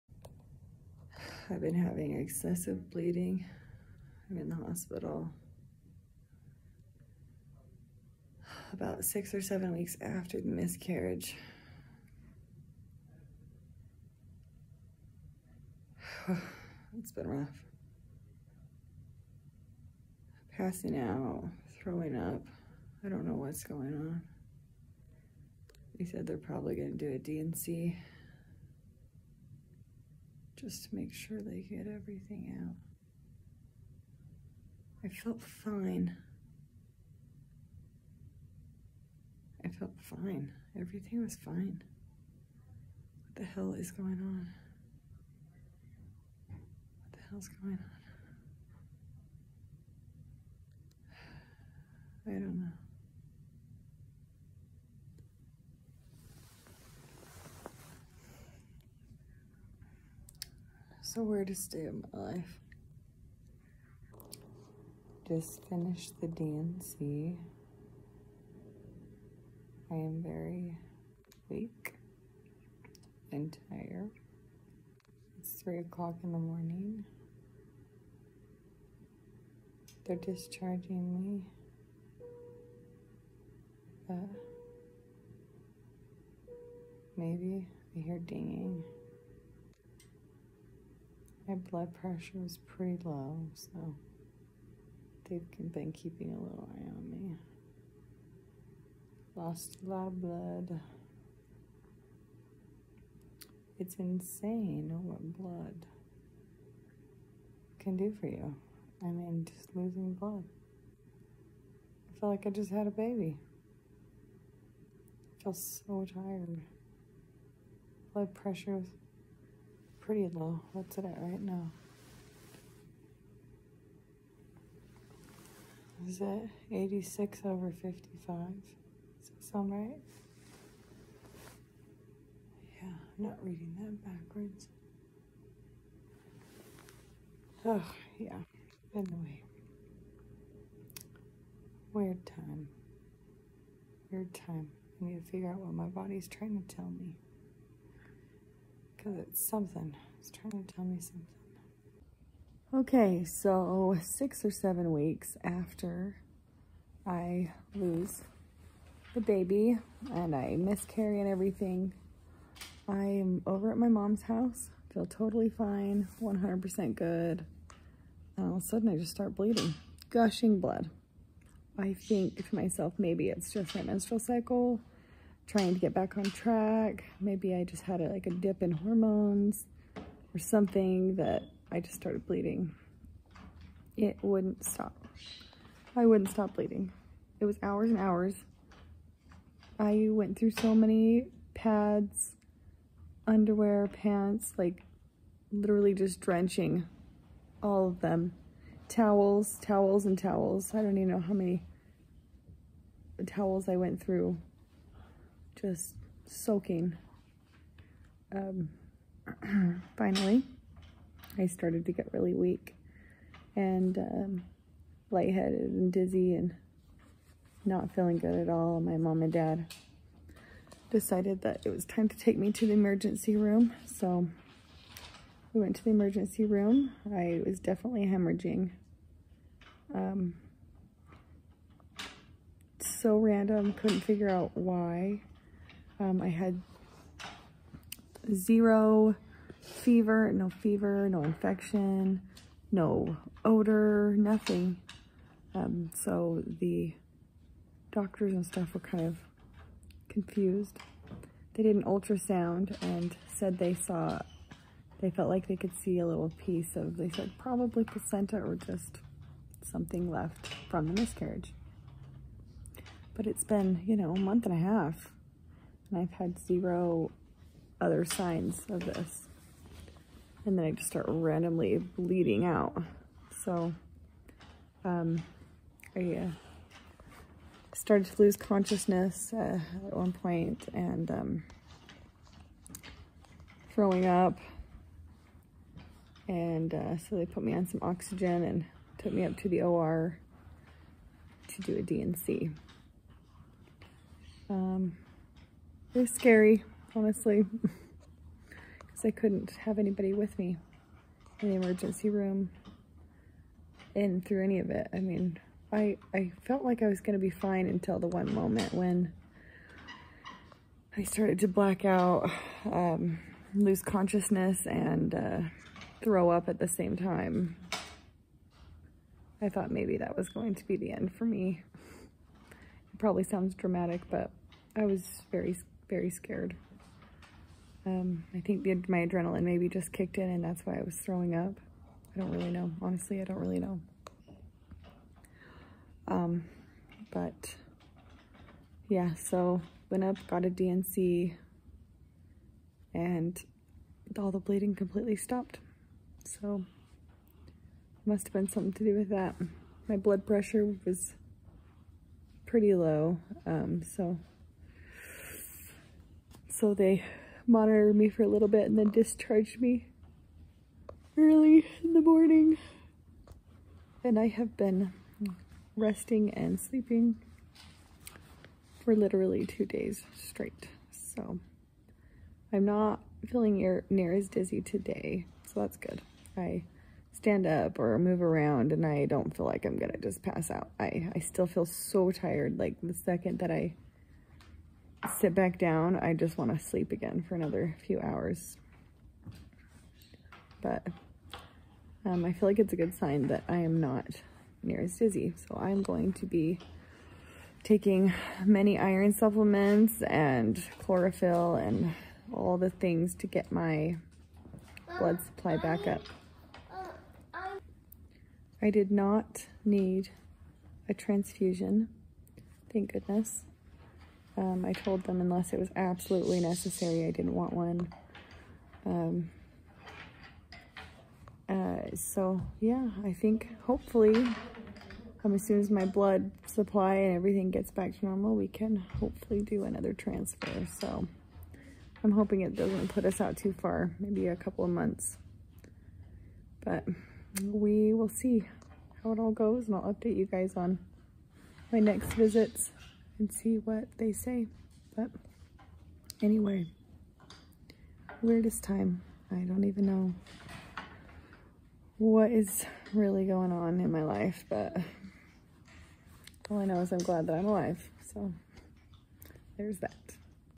My gestational surrogacy journey video # ?
Hospitalized for hemorrhaging six weeks after miscarriage.